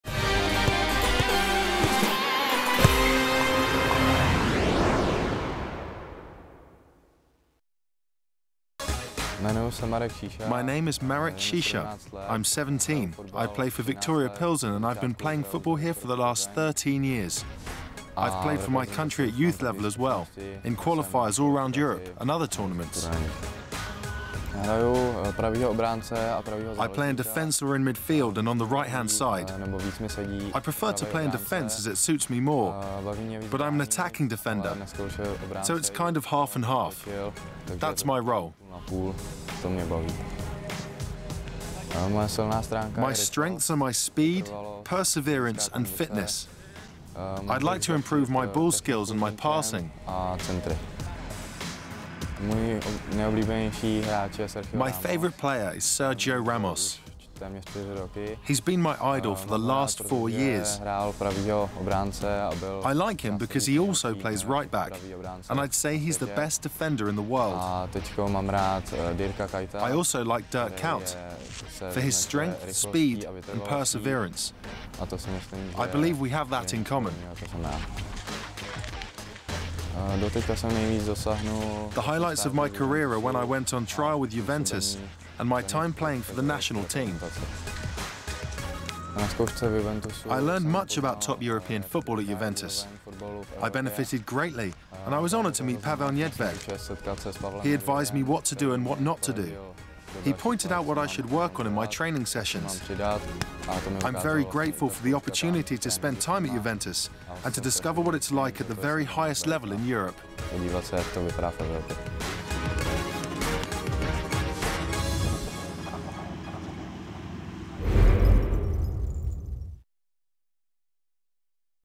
Young Talent: Marek Sisa
A promising Czech Republic defender talks about his playing style, his admiration for a Spanish international and more.